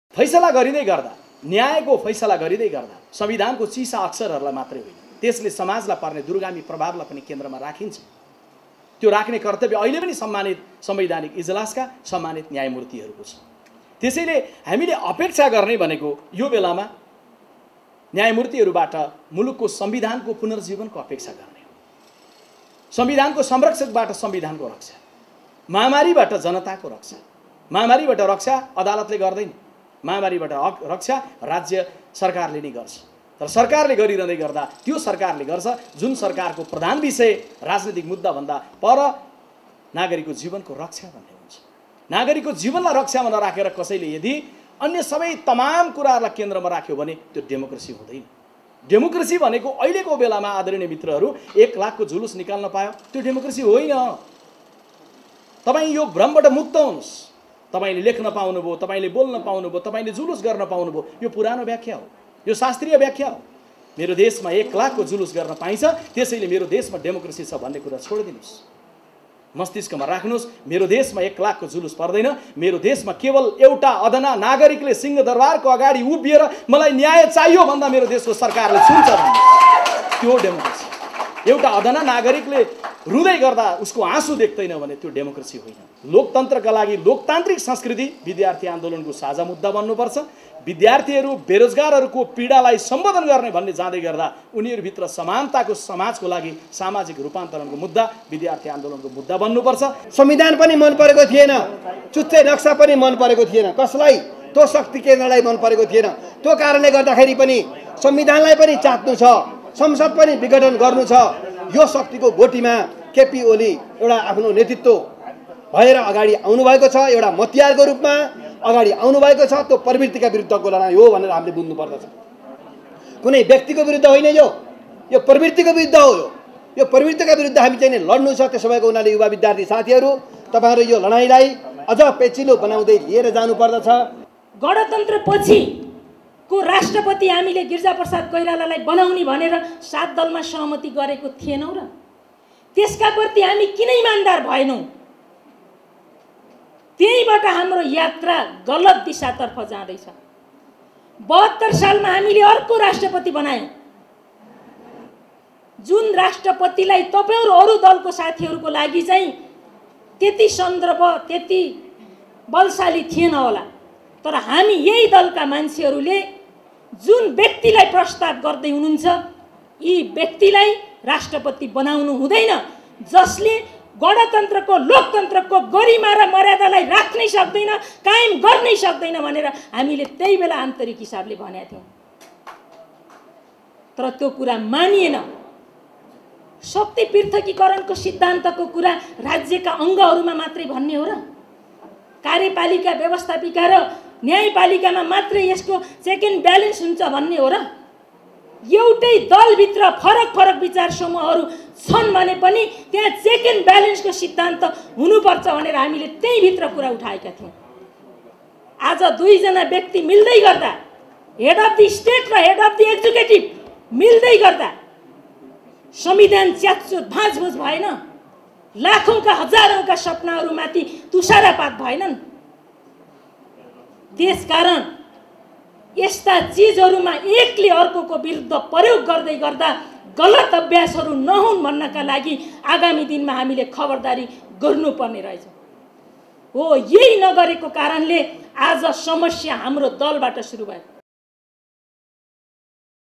विपक्षी गठबन्धनका युवा नेताको अपेक्षा : अदालतले संविधानलाई पुनर्जीवन दिन्छ
- विपक्षी गठबन्धनका विद्यार्थी संगठनले आयोजना गरेको संसद विघटन, अधिनायकवाद र विद्यार्थी आन्दोलन विषयक अन्तरक्रियामा